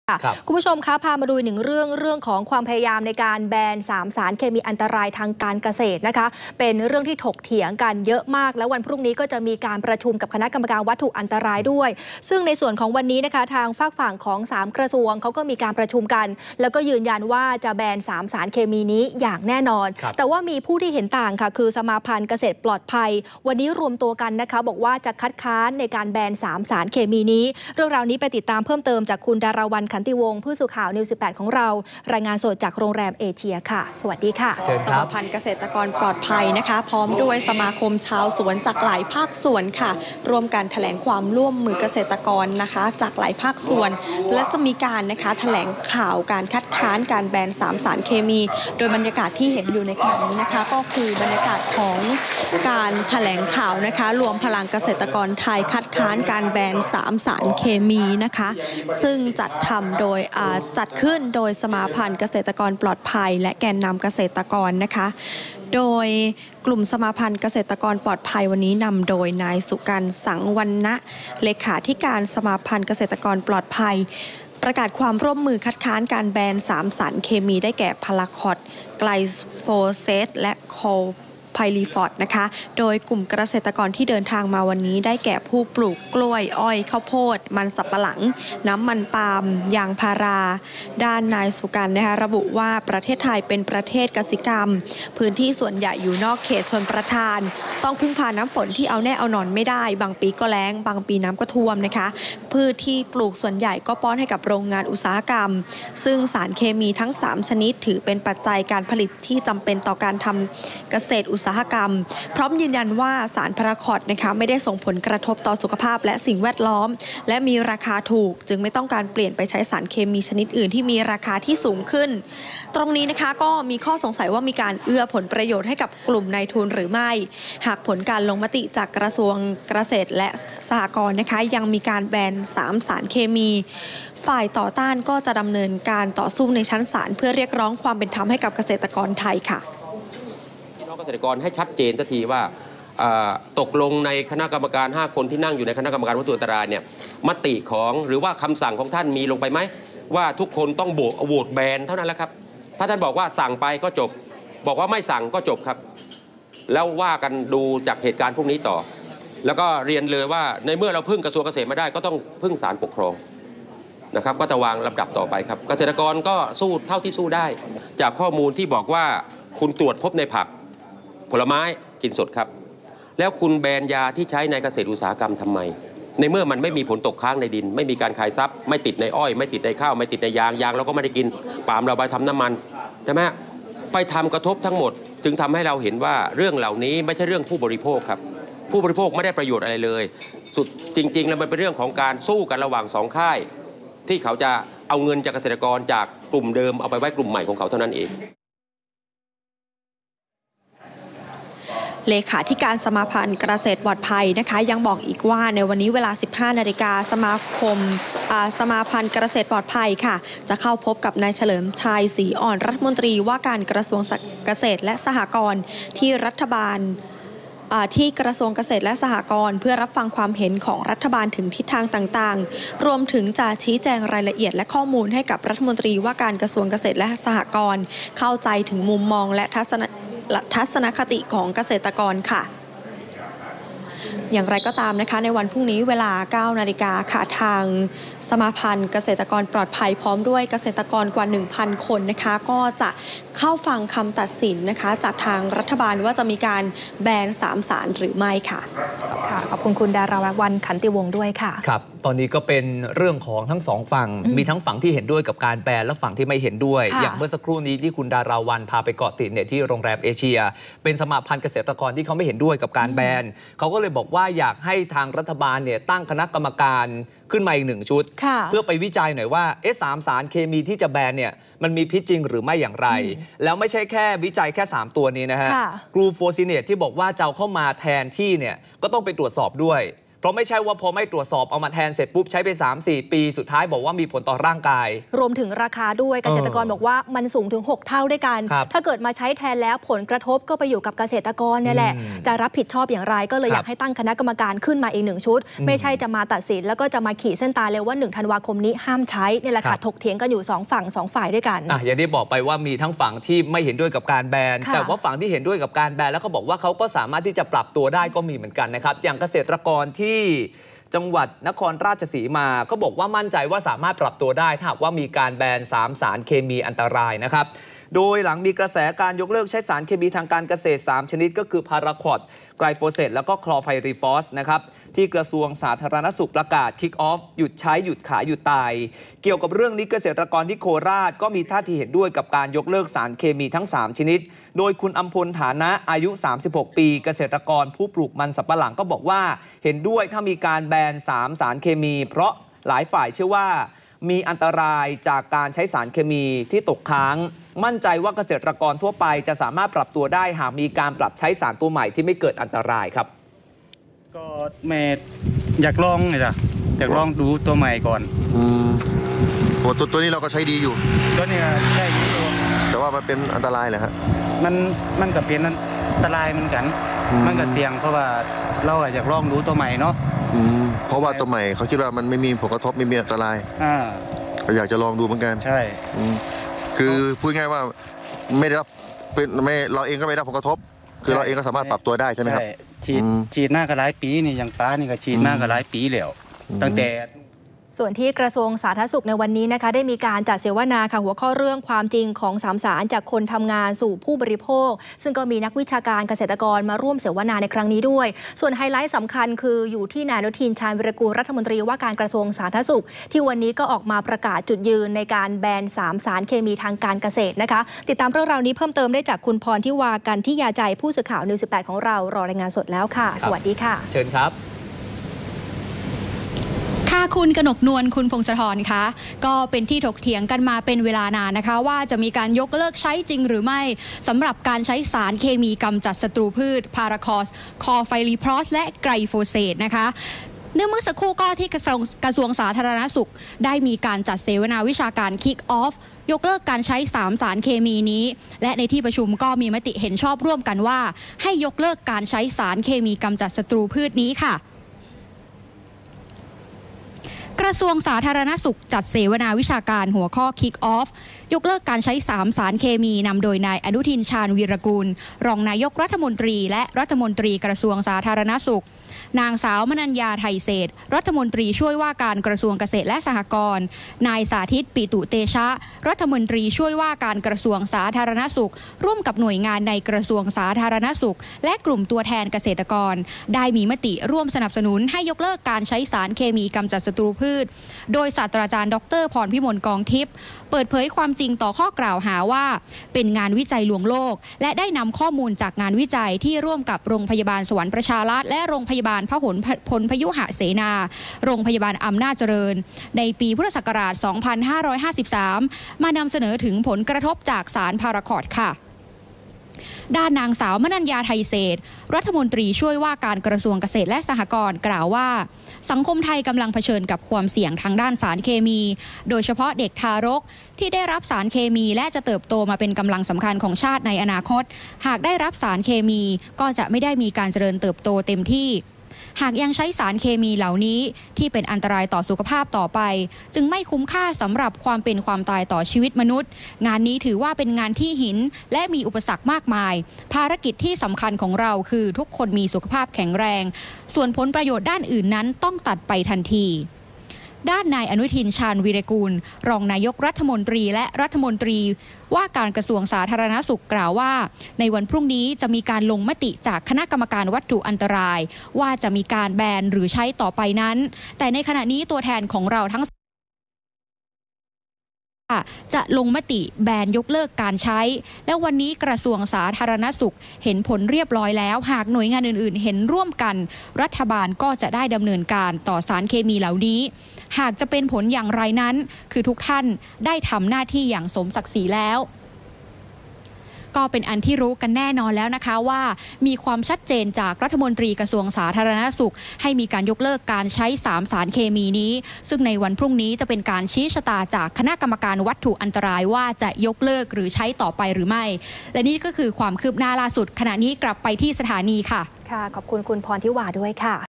สมาพันธ์เกษตรฯ ฮือค้านแบนสารเคมี | NEW18
จากกรณีที่รัฐมนตรี 3 กระทรวง ประกอบด้วย กระทรวงสาธารณสุข กระทรวงเกษตรและสหกรณ์ และกระทรวงอุตสาหกรรม เห็นพ้องต้องกันให้มีการแบน 3 สารเคมีที่ใช้ในการเกษตร ประกอบด้วย พาราควอต ไกลโฟเซต คลอร์ไพริฟอส จึงกลายเป็นร้อนขึ้นมาทันที

อ่านข่าวเพิ่มเติมได้ที่